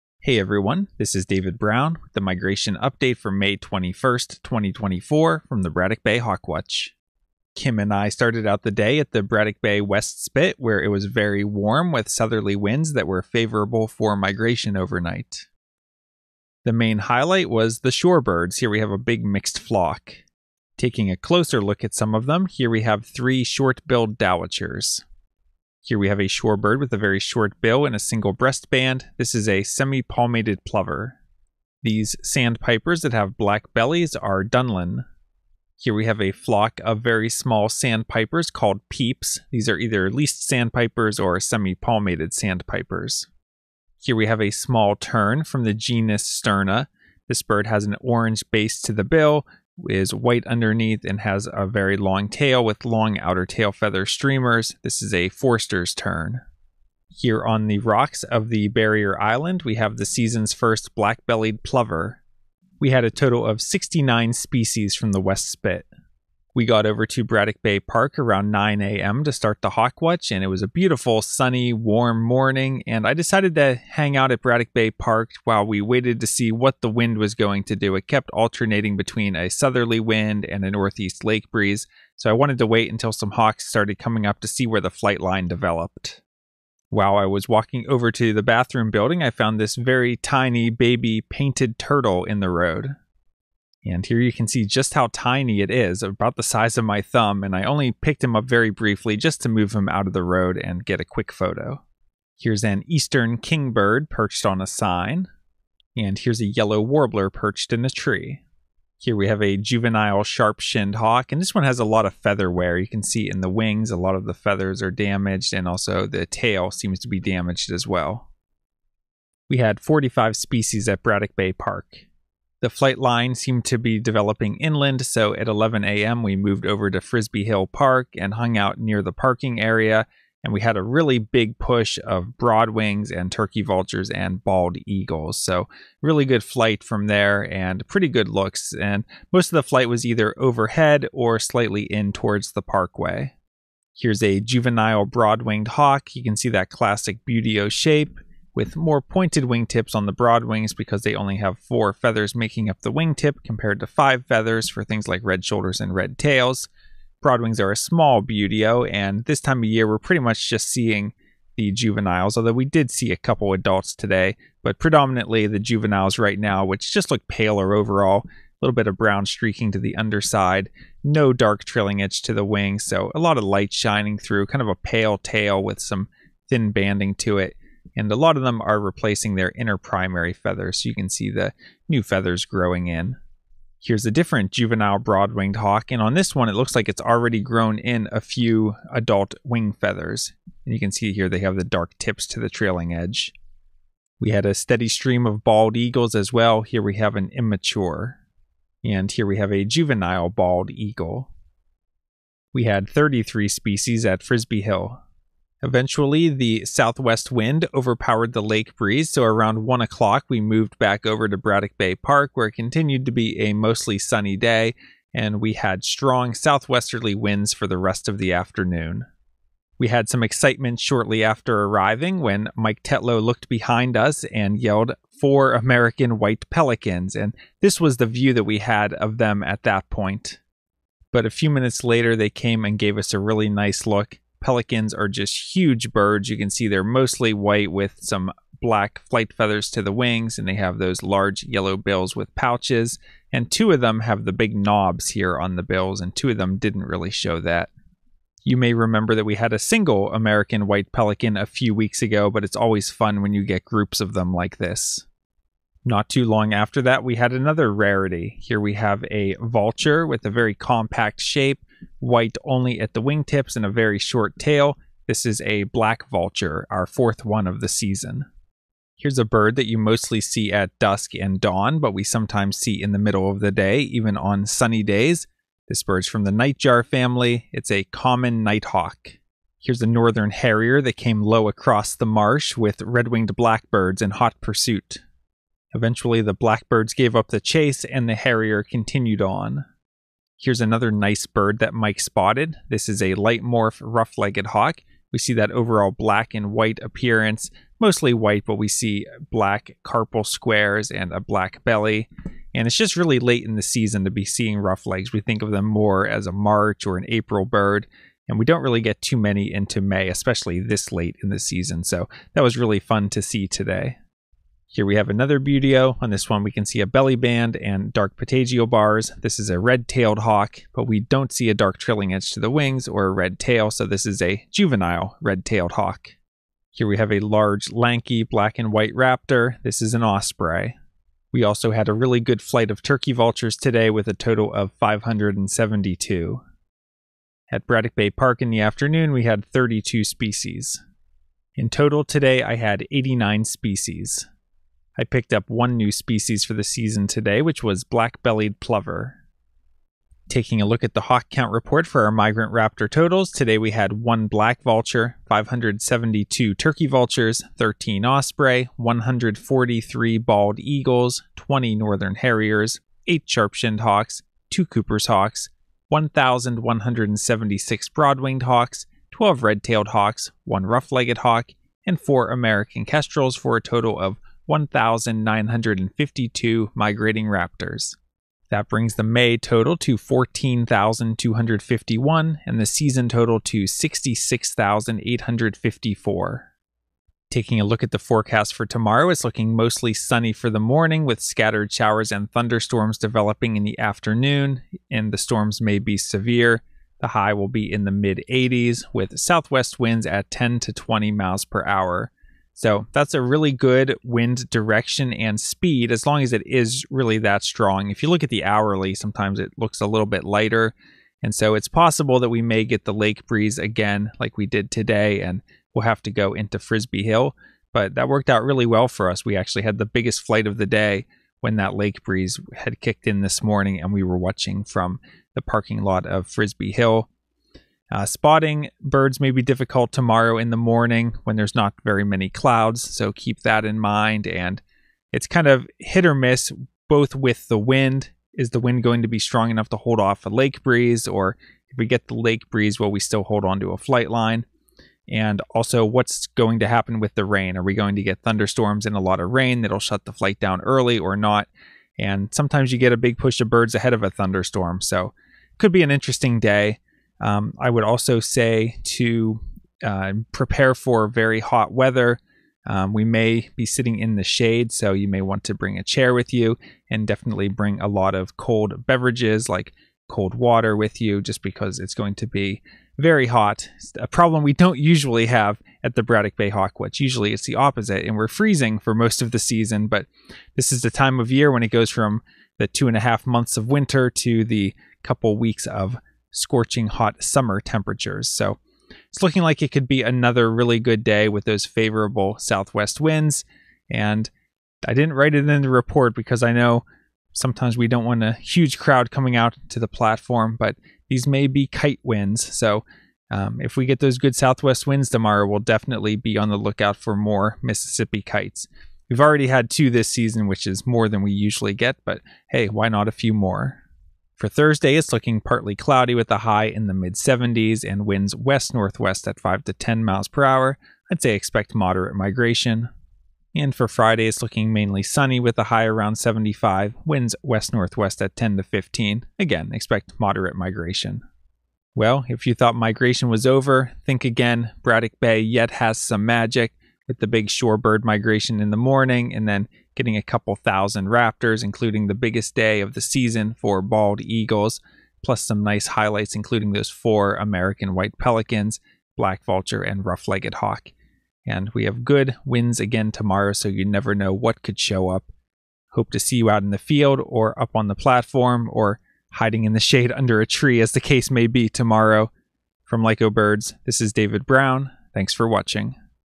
[82] Four White Pelicans, Black Vulture, Rough-legged Hawk at the Braddock Bay Hawk Watch, 5/21/24
Hawk counter David Brown gives the daily hawkwatching update from the Braddock Bay Hawk Watch in Rochester, NY, USA.